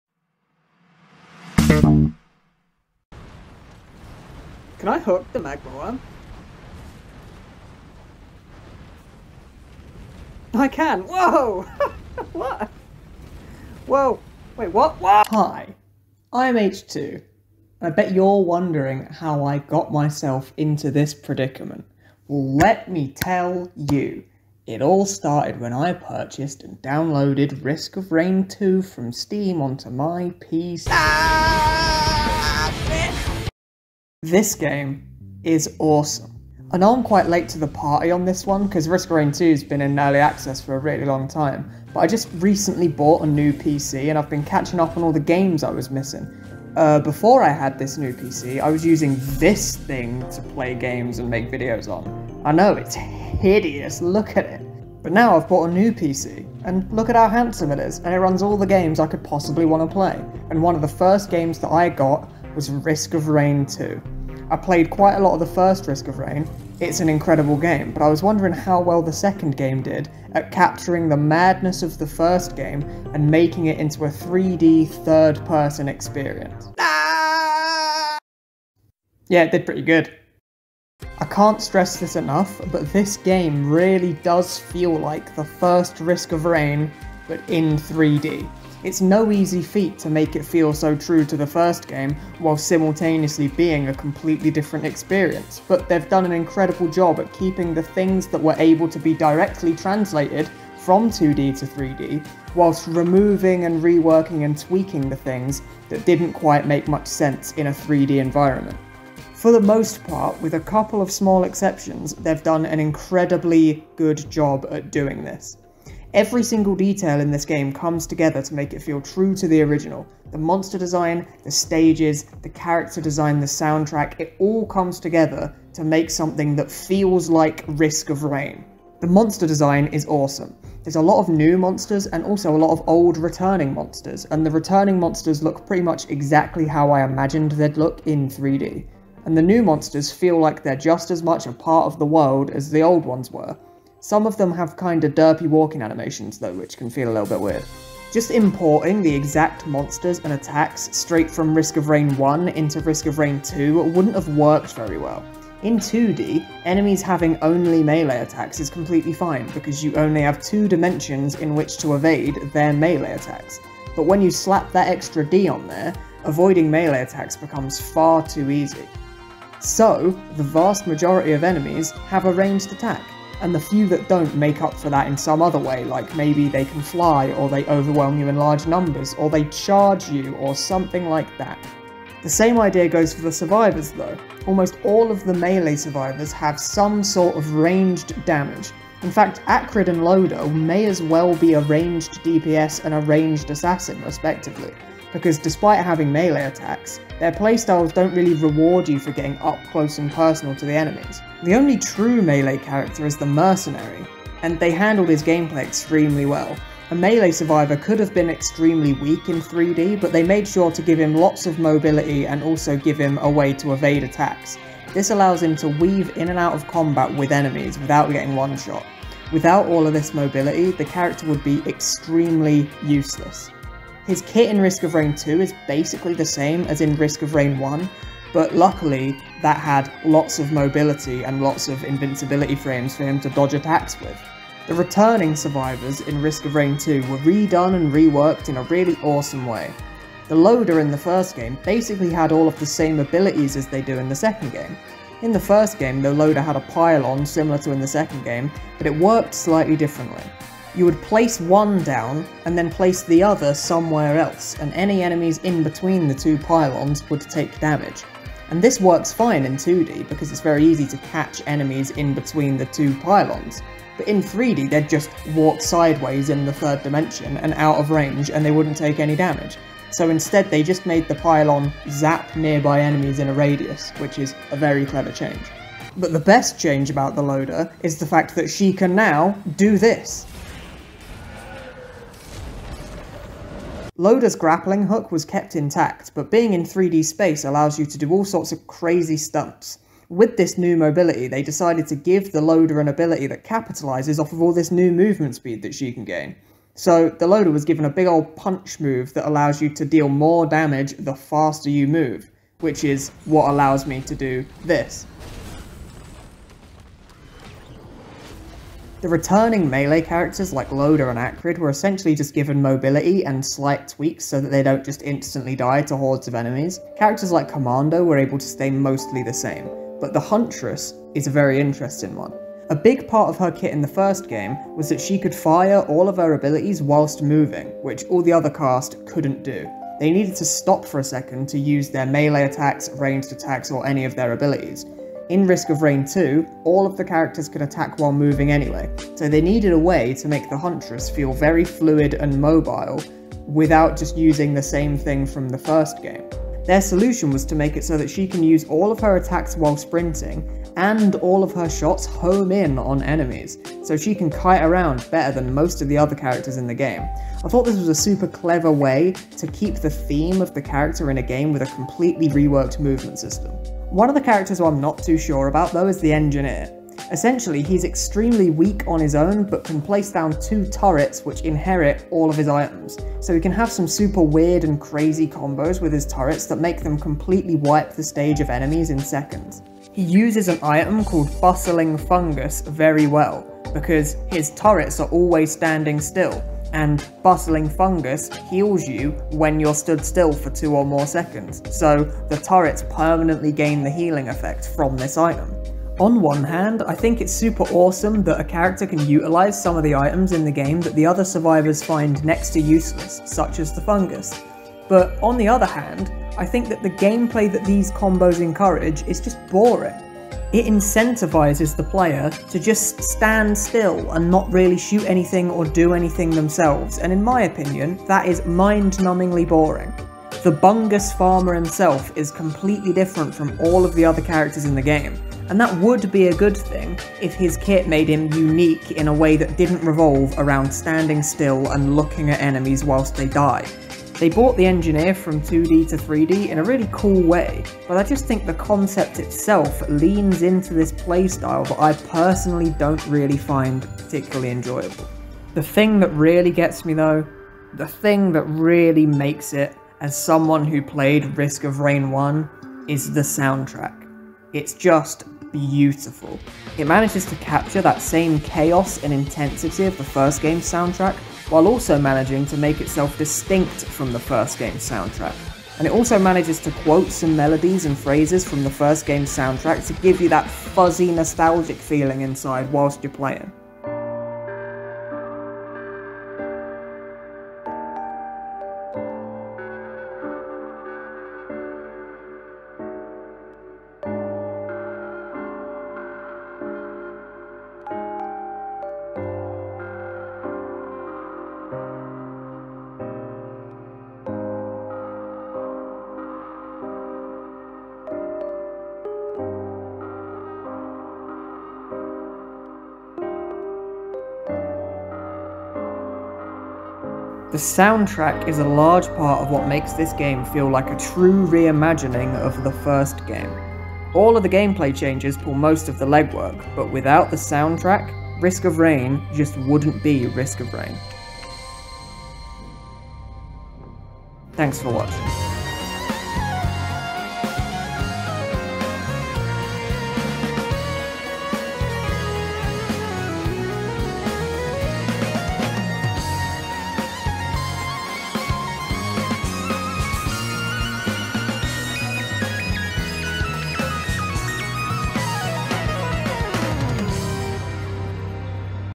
Risk of Rain's Transition From 2D to 3D
Risk of Rain 2 really does feel like the first game but made into 3d, so today I talked about how they achieved this